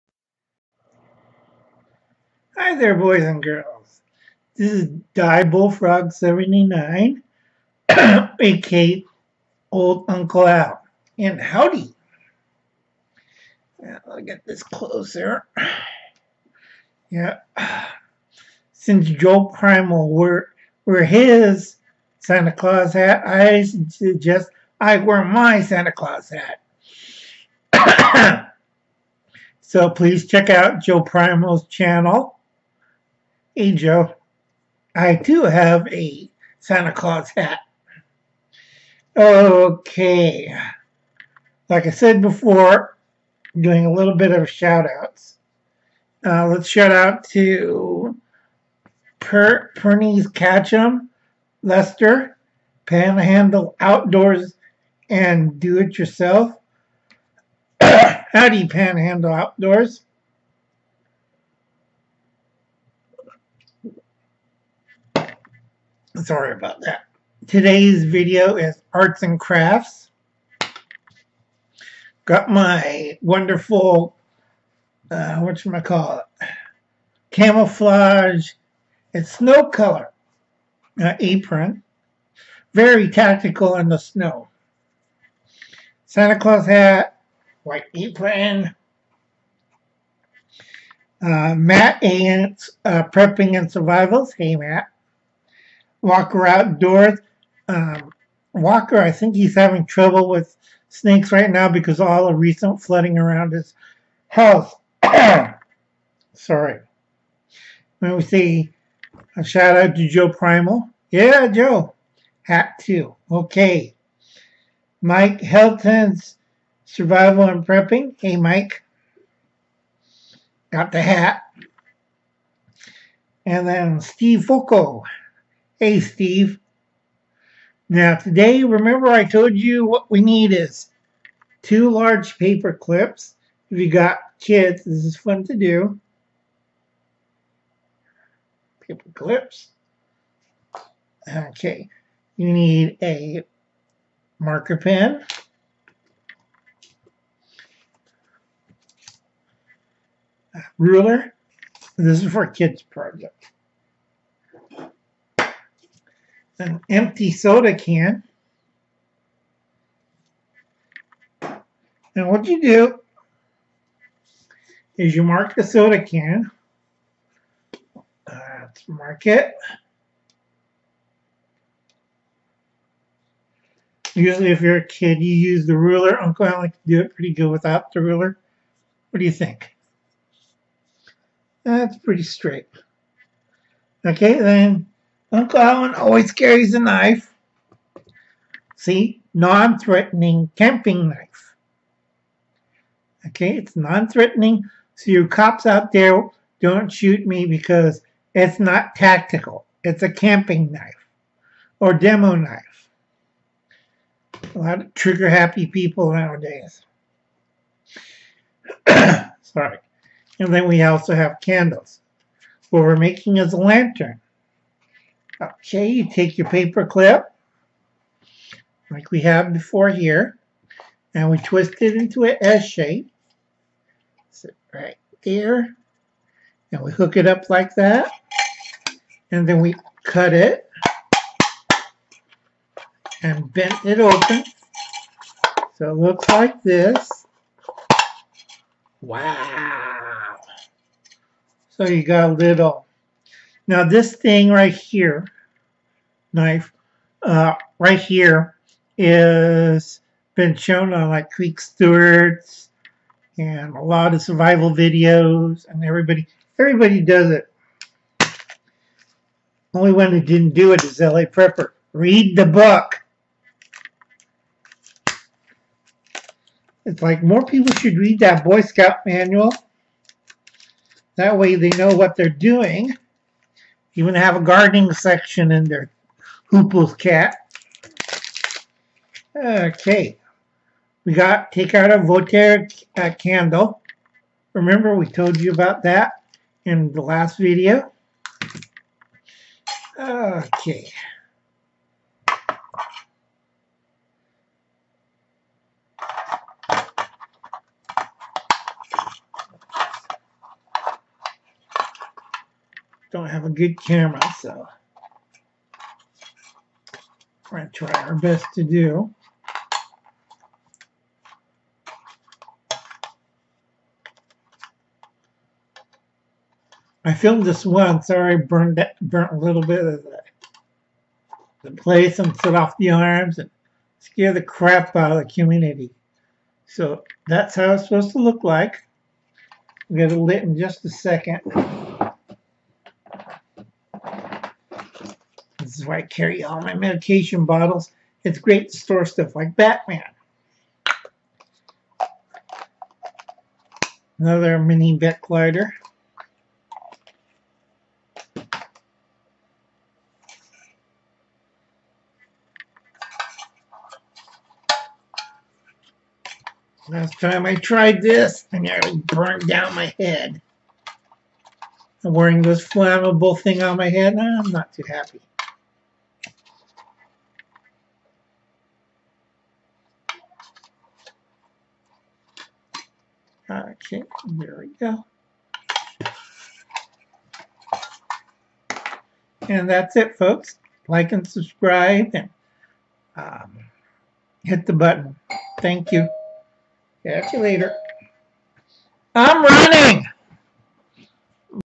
Diebulfrog79 - Arts and Crafts - Soda can lantern
Something a little strange and something a little weird. Not your normal YouTube channel. Beyond the doorway. Wacky Arts and crafts - Soda can lantern. From an empty soda can, paper clip, and a candle. Have fun with Uncle Al.